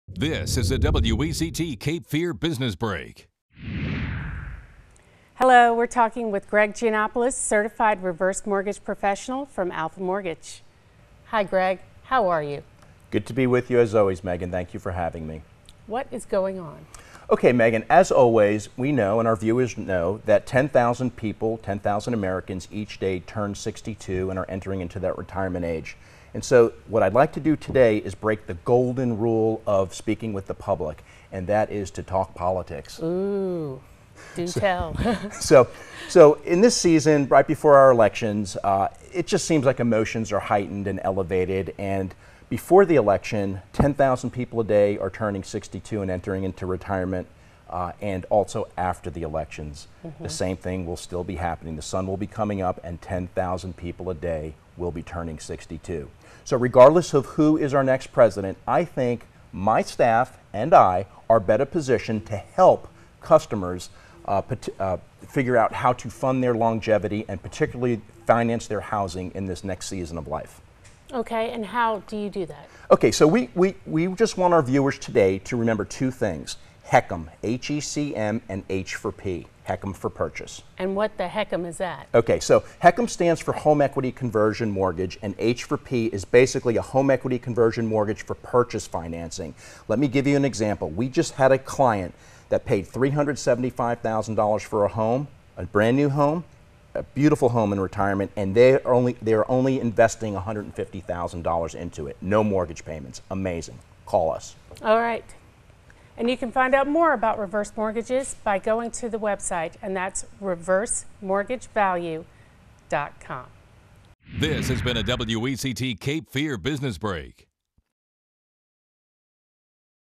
HECM and H4P Options for Retirees - Alpha Mortgage
Transcription:

Meghan: Hello, we are talking with Greg Gianoplus, Certified Reverse Mortgage Professional, from Alpha Mortgage. Hi Greg, how are you?

Greg: Good to be with you as always, Meghan, thank you for having me.

Meghan: What is going on?

Greg: OK Meghan, as always, we know and our viewers know that 10,000 people, 10,000 Americans, each day turn 62 and are entering into that retirement age. And what I would like to do today is break the Golden Rule of speaking with the public and that is to talk politics.

Meghan: Do tell.

Greg: So in this season right before our elections, it seems like emotions are heightened and elevated, and before the election 10,000 people a day are turning 62 and entering into retirement, and also after the elections the same thing will be happening; the sun will be coming up and 10,000 people a day will be turning 62. So regardless of who is our next president, I think my staff and I are better positioned to help customers figure out how to fund their longevity and particularly finance their housing in this next season of life.

Meghan: OK, and how do you do that?

Greg: We just want our viewers today to remember two things: HECM and H4P, HECM for purchase.

Meghan: And what the HECM is that?

Greg: So HECM stands for Home Equity Conversion Mortgage and H4P is basically a Home Equity Conversion Mortgage for purchase financing. Let me give you an example. We just had a client that paid $375,000 for a home, a brand new home - a beautiful home in retirement - and they are only investing $150,000 into it, no mortgage payments. Amazing. Call us.

Meghan: Alright. And you can find out more about reverse mortgages by going to the website and that's reversemortgagevalue.com.

The Reverse Mortgage Advisors at Alpha Mortgage are some of the most knowledgeable and highly trained professionals in the marketplace.